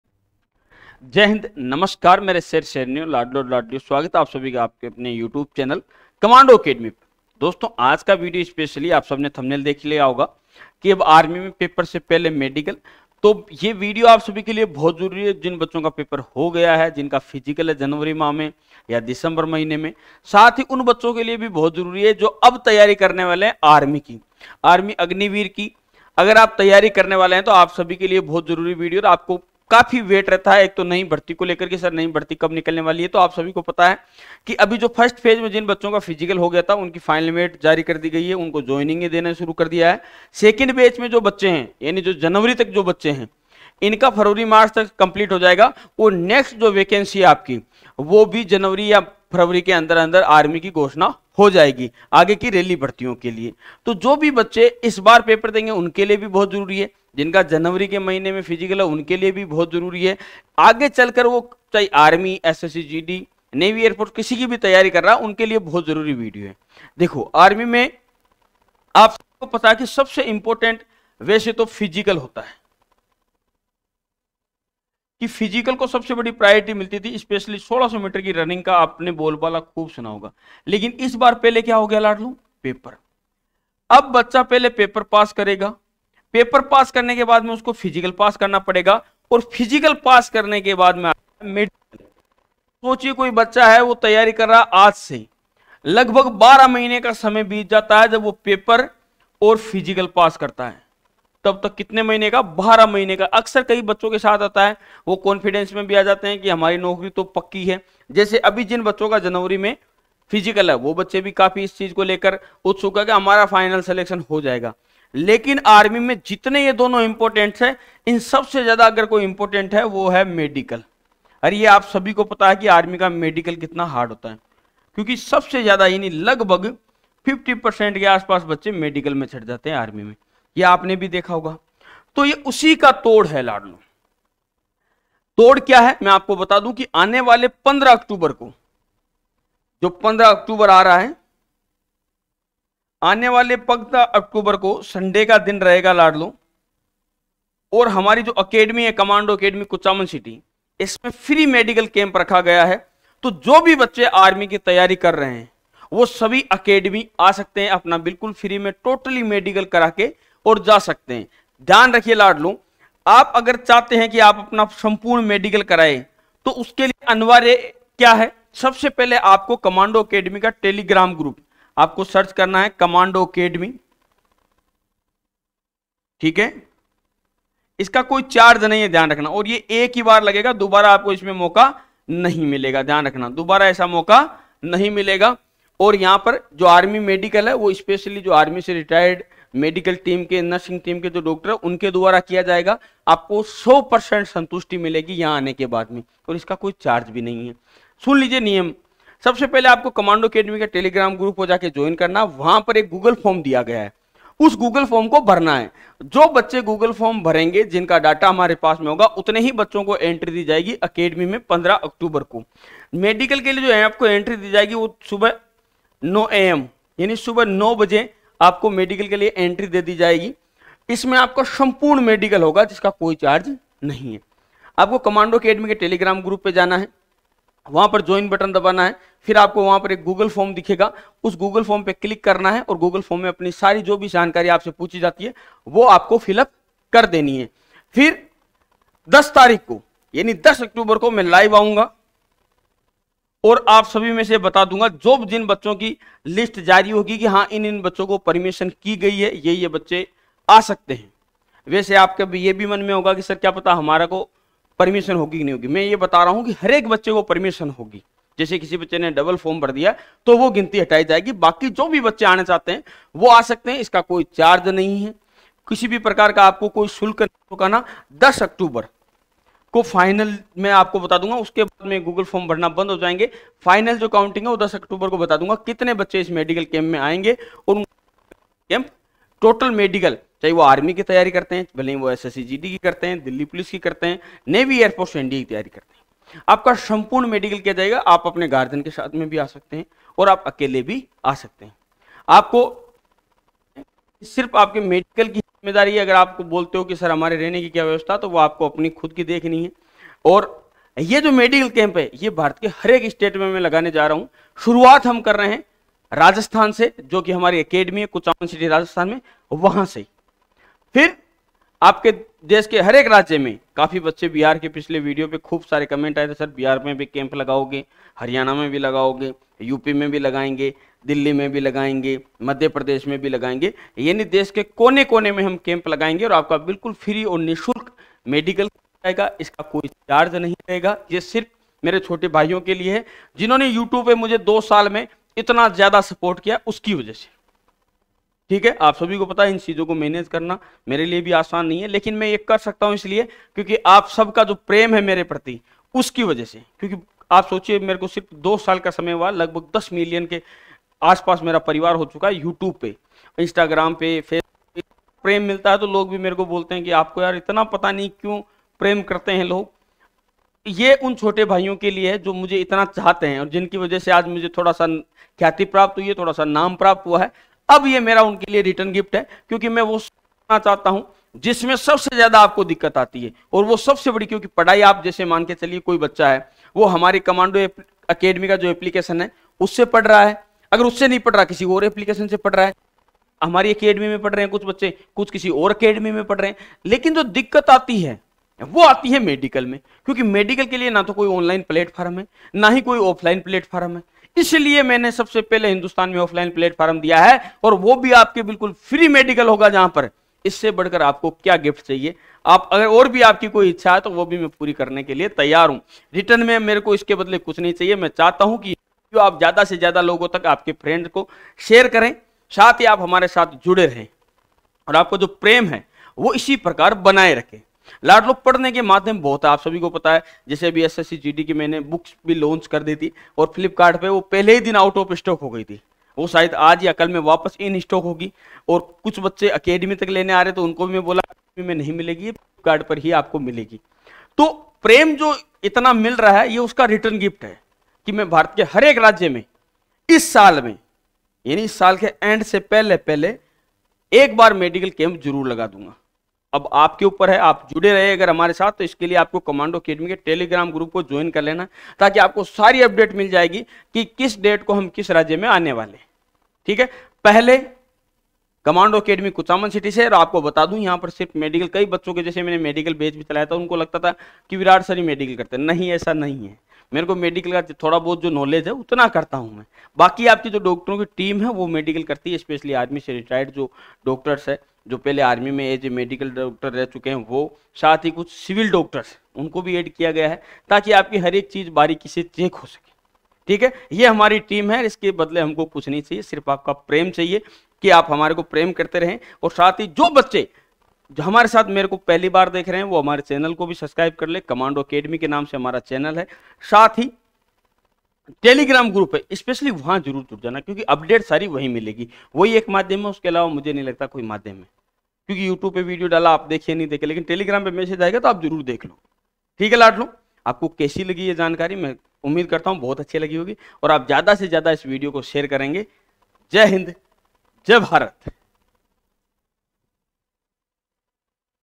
big update || indian army agniveer new vacancy | पेपर से पहले मेडिकल ?
3 Month Batch (3 महीने का बैच)

Virat Choudhary
CRPF Commando